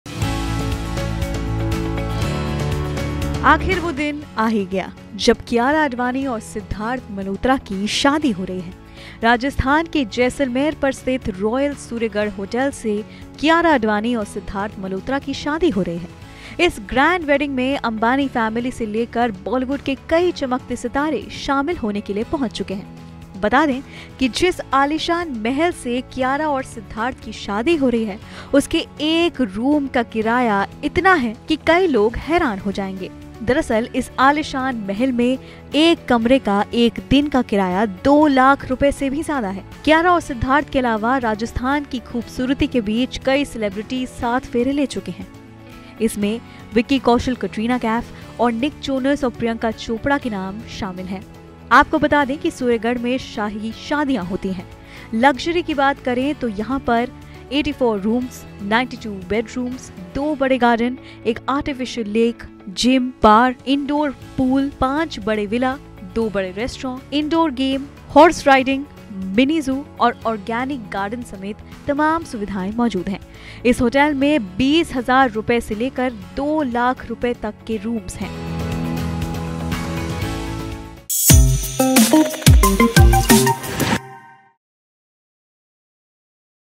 Sidharth Malhotra-Kiara Advani wedding: सूर्यगढ़ पैलेस के कमरे का कितना किराया है?, होश उड़ जाएंगे
राजस्थान के जैसलमेर पर स्थित रॉयल सूर्यगढ़ होटल से कियारा आडवाणी और सिद्धार्थ मल्होत्रा की शादी हो रही है. इस ग्रैंड वेडिंग में अंबानी फैमिली से लेकर बॉलीवुड के कई चमकते सितारे शामिल होने के लिए पहुंच चुके हैं. आपको बता दें कि जिस आलीशान महल से कियारा और सिद्धार्थ की शादी हो रही है  उसके एक कमरे का एक रात का किराया 2 लाख रूपए तक है.